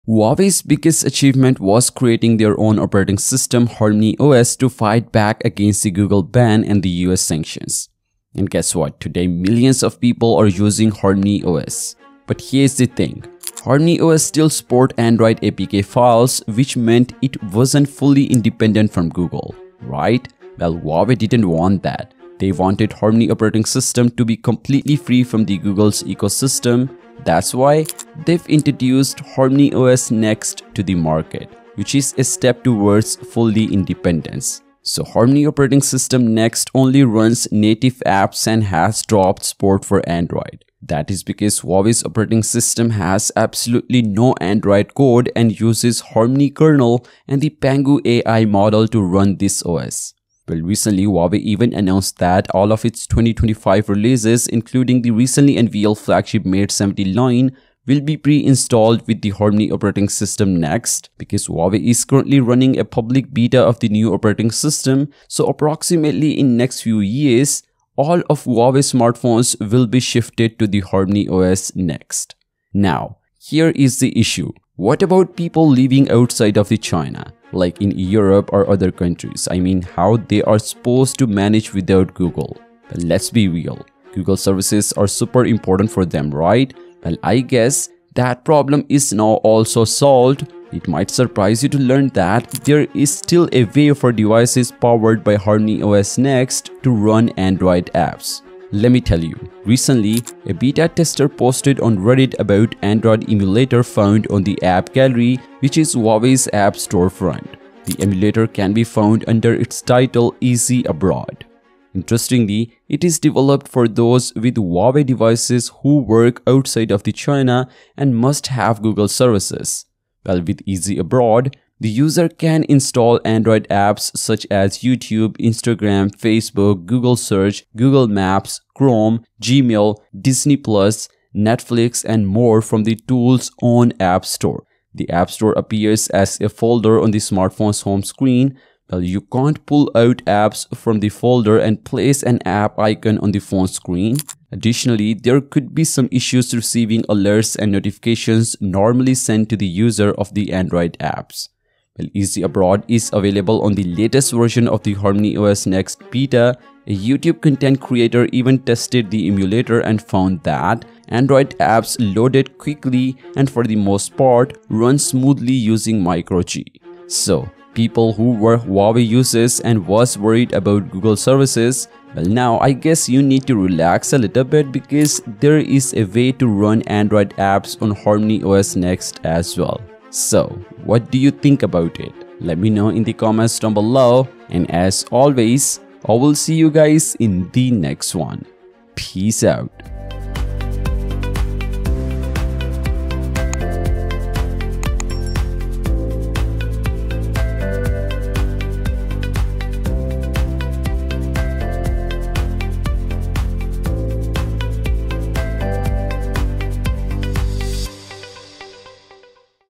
Huawei HarmonyOs NEXT - Finally Fixing the BIGGEST PROBLEM !!
HEY GUYS,
So, Huawei’s biggest achievement was creating their own operating system, HarmonyOS, to fight back against the Google ban and U.S. sanctions. And guess what? Today, millions of people are using HarmonyOS!
But here’s the thing—HarmonyOS still supported Android APK files, which meant it wasn’t fully independent from Google. Huawei didn’t want that. They wanted HarmonyOS to be completely free from Google’s ecosystem. That’s why they’ve introduced HarmonyOS Next to the market—a step towards full independence.
So, HarmonyOS NEXT operating system only runs native apps and has dropped support for Android. That is because, Huawei's operating system has absolutely no Android code and uses the Harmony kernel, and the Pangu AI model to run this OS.
Well, Recently Huawei announced that all of its 2025 releases, including the recently unveiled flagship Mate 70 line, will be pre-installed with HarmonyOS Next. Huawei is currently running a public beta of the new operating system, so approximately in next few years all of huawei smartphones will be shifted to HarmonyOs next? But here’s the issue—what about people living outside of China, like in Europe or other countries? I mean, how are they supposed to manage without Google? Let’s be real, Google services are super important for them, right? Well I guess that problem is not solved as well. It might surprise you to learn that there is a way for devices powered by HarmonyOS NEXT to run Android apps.

PEACE OUT!!